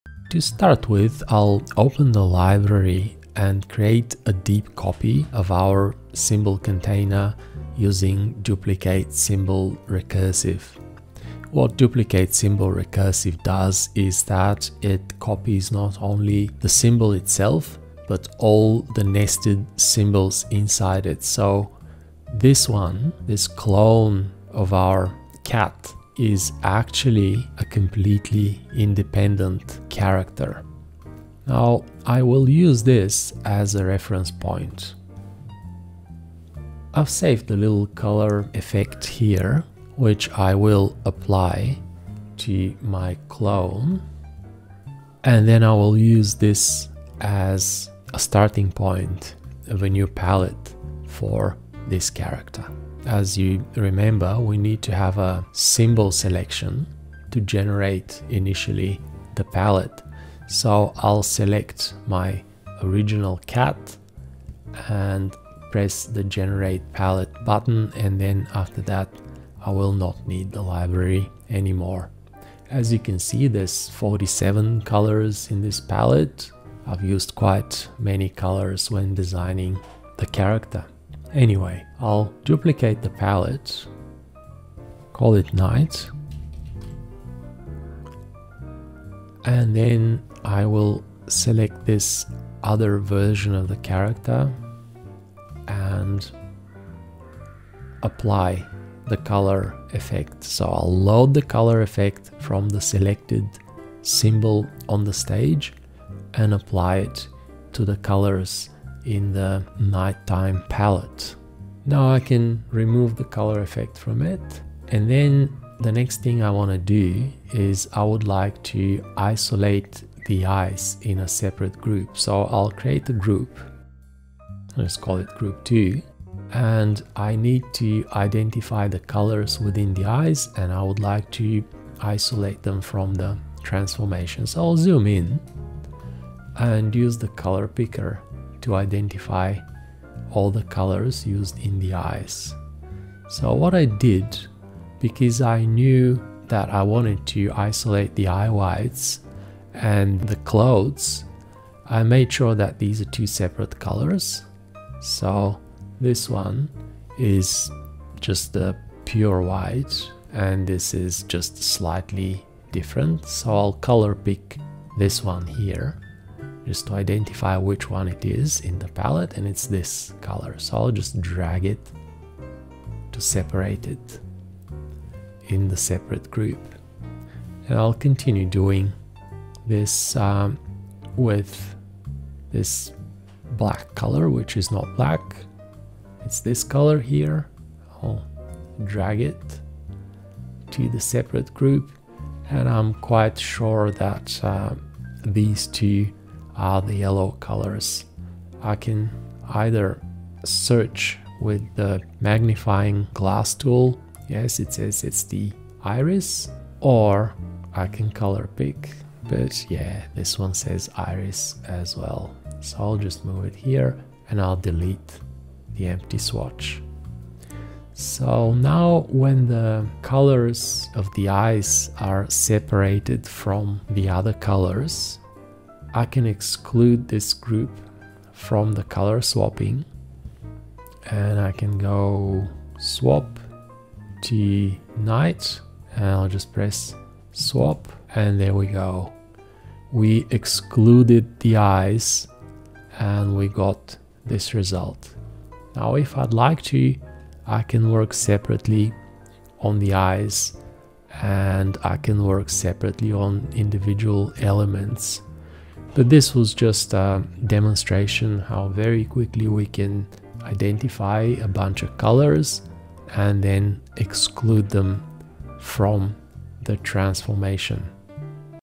Adobe Animate - Creating a night-time palette with Symbol Palette Control
Symbol Palette Control is an interactive panel which allows users to create multiple color palettes associated with a Symbol and then switch between these palettes.
Color palettes can be copied (cloned), modified with color effects, reorganized, saved as files or loaded from a saved file.
Palettes can also be optimized, by unifying the color of similar swatches, thus reducing the overall number of colors.
Color swatches can be modified individually, grouped, regrouped or excluded from color swapping.

Presented by Nickolay Tilcheff